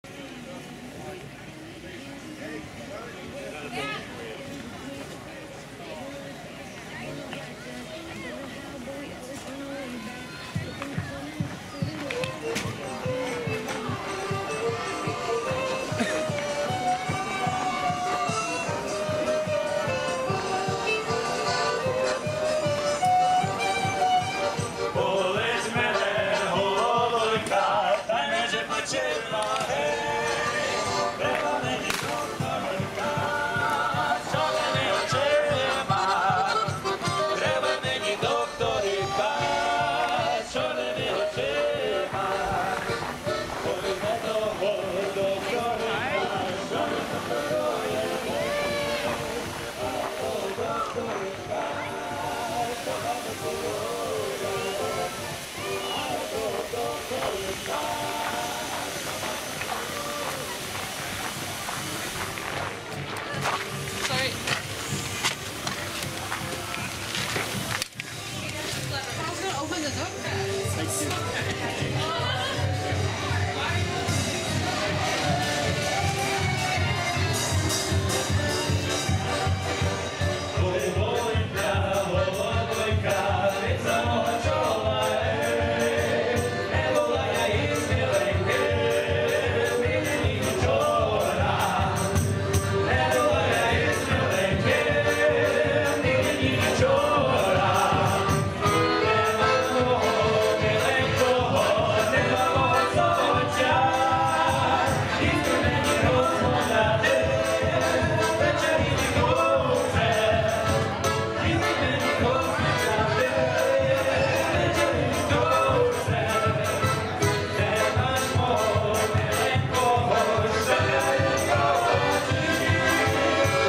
Edmonton Deep Freeze Festival
Just a very small clip of this festival on the closed down 118 Avenue in Edmonton, Alberta. There was a whole lot more going on, than this video clip shows. It was Ukrainian day, the music heard outside was actually being performed live, featuring Zabava U Koli from inside the Alberta Ave Community Centre, which is shown on the 2nd half of the video, as I walk in from the outside.